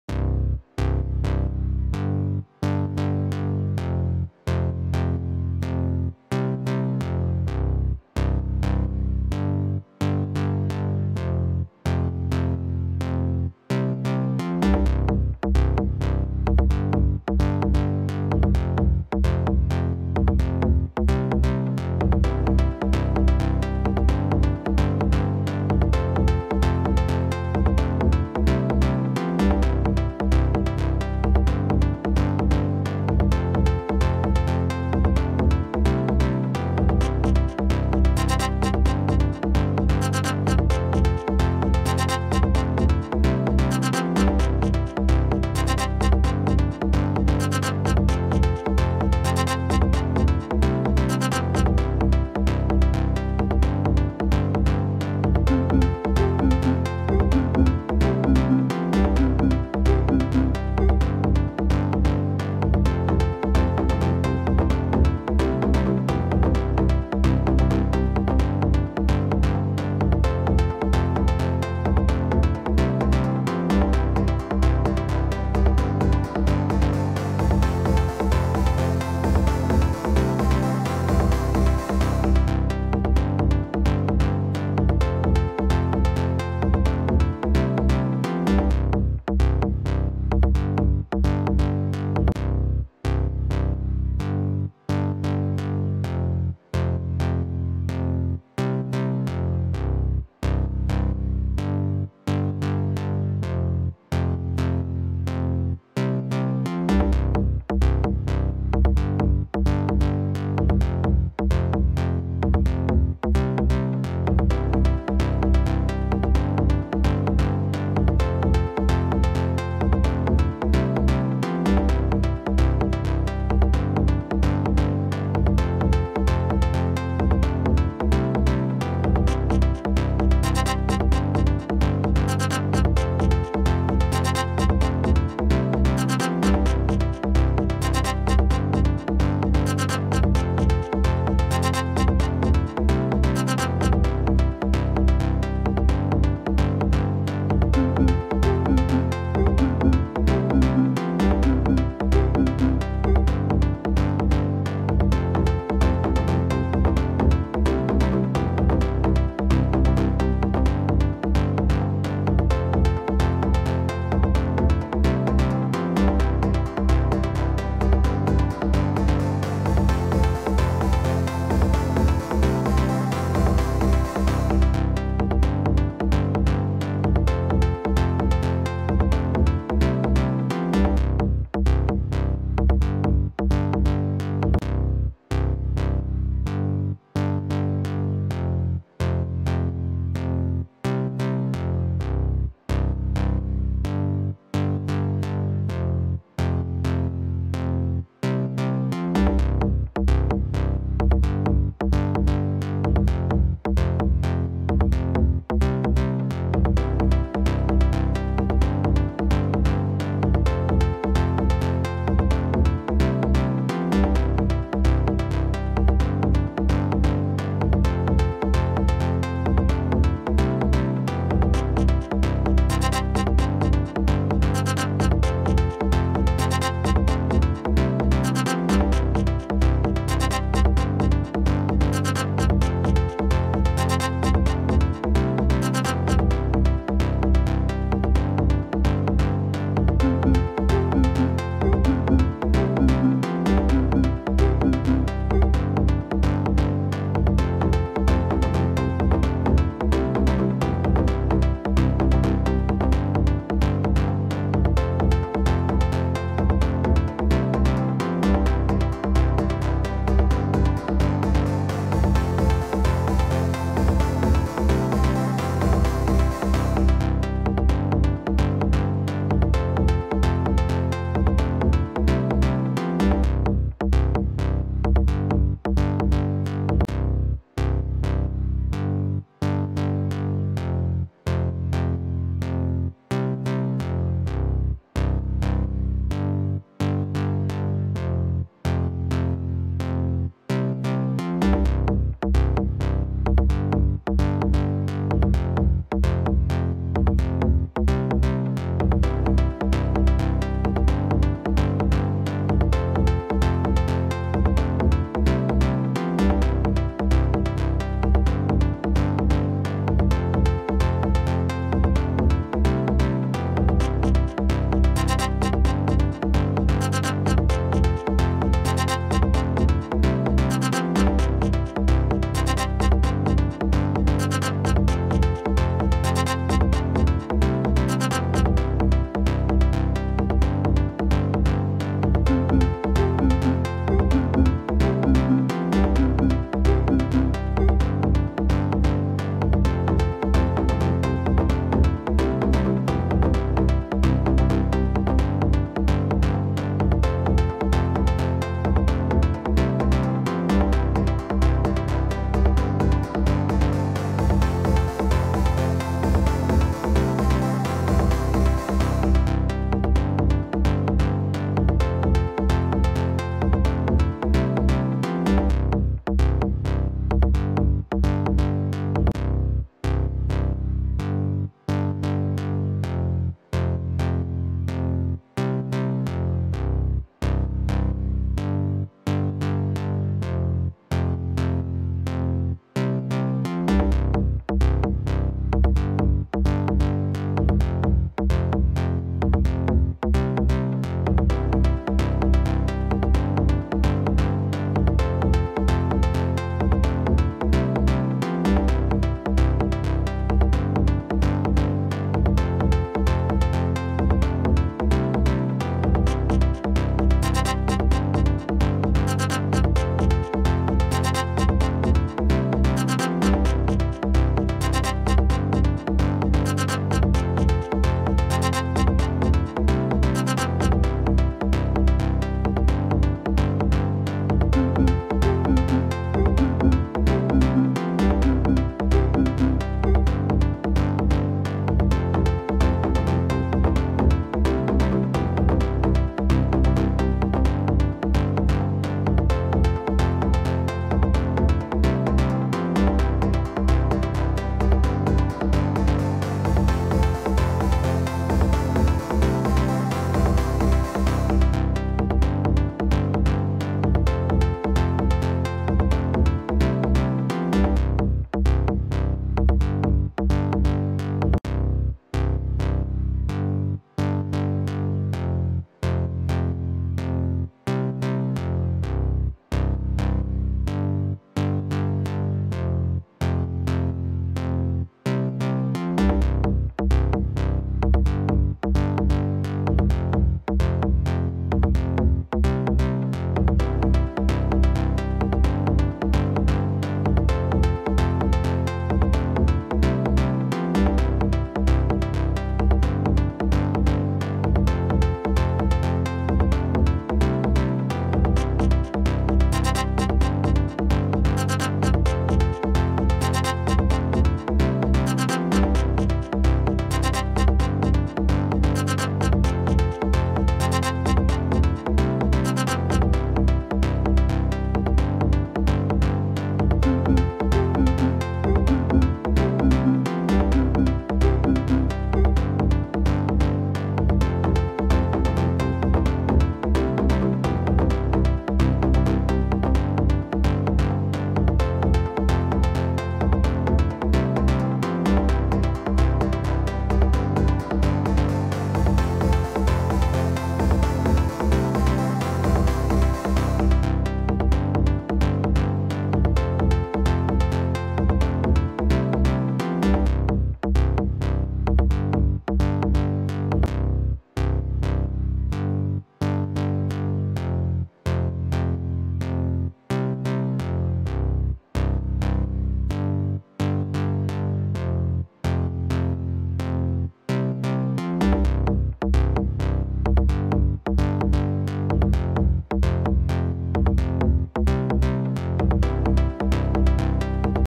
WPL B36 semi-trailer with crane body Functional test must see
Easy to build conversion of a B36 Ural to the tractor with loading crane from two together screwed Servos.The step by step conversion video I'll show you as the next video is curious and subscribe so you do not miss it